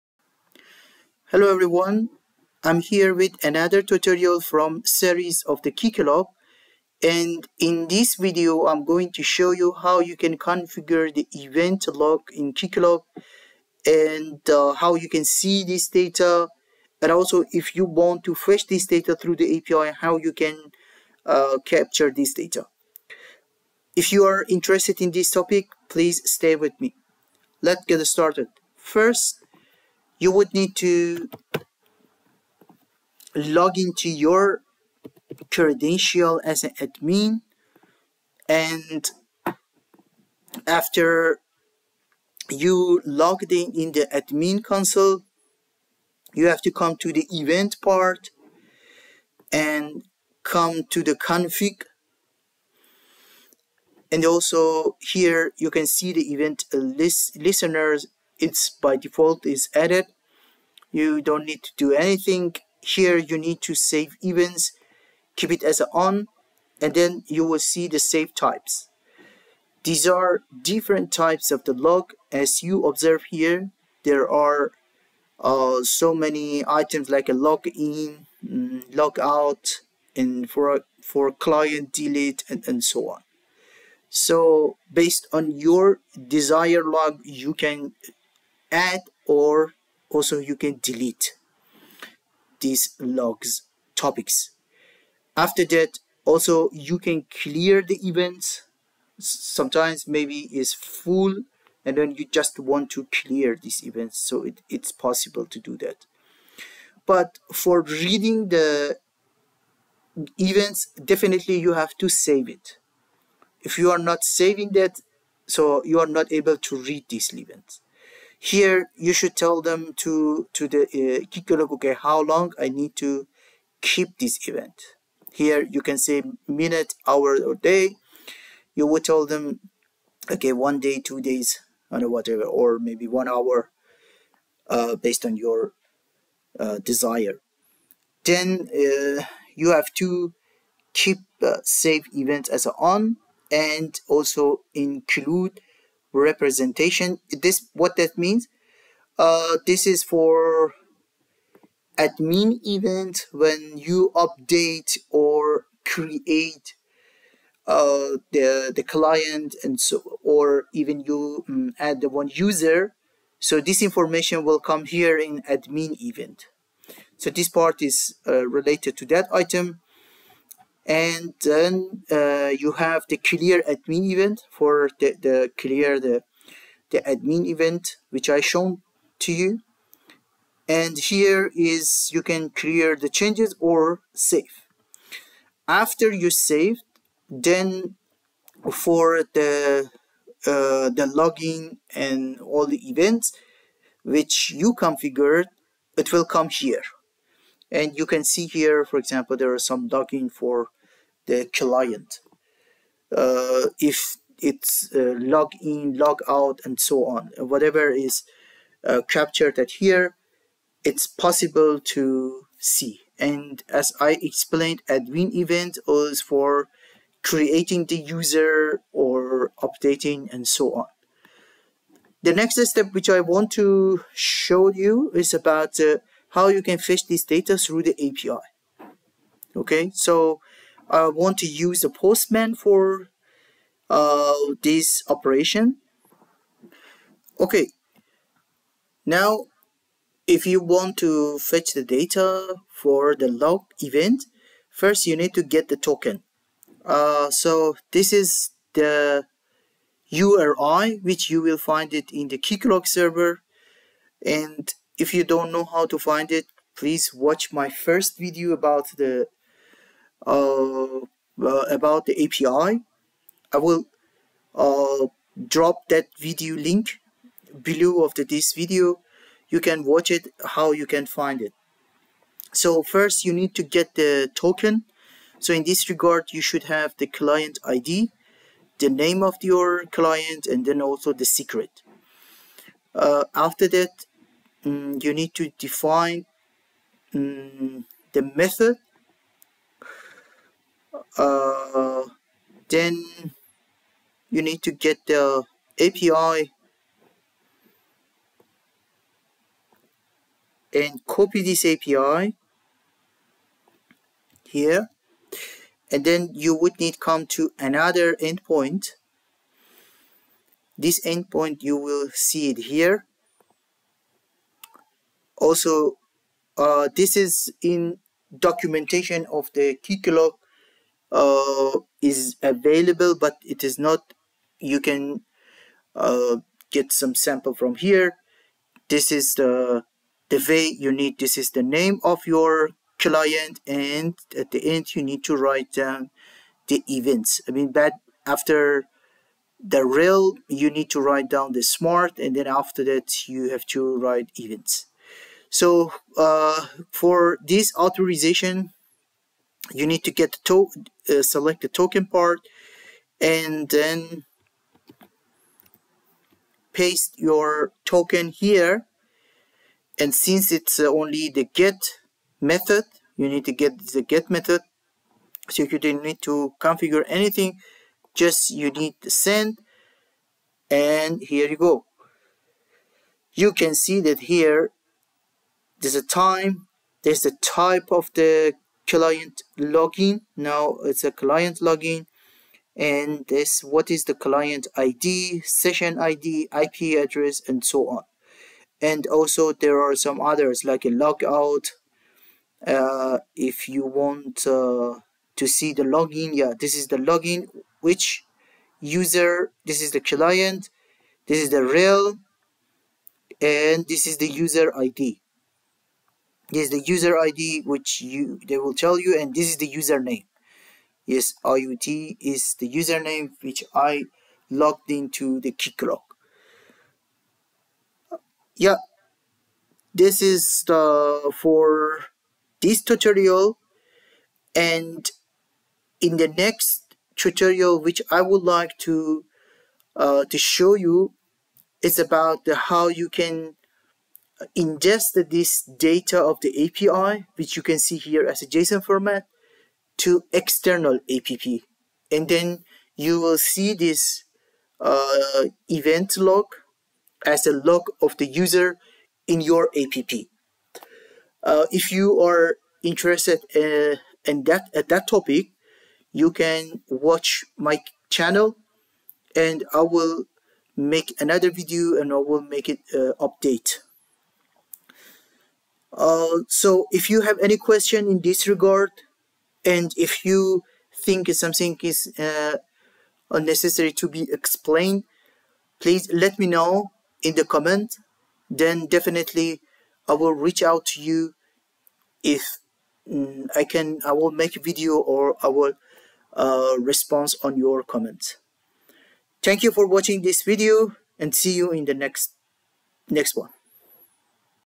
Keycloak Event Logging| Step-by-Step Guide to Configuration and API Data Retrieval
🔐 Dive into the world of Keycloak Event Logging with our comprehensive tutorial! Learn how to configure event logs and harness the power of Keycloak's API to fetch critical data. Whether you're a developer, administrator, or security enthusiast, this step-by-step guide will empower you to efficiently monitor and extract valuable insights from your Keycloak instance.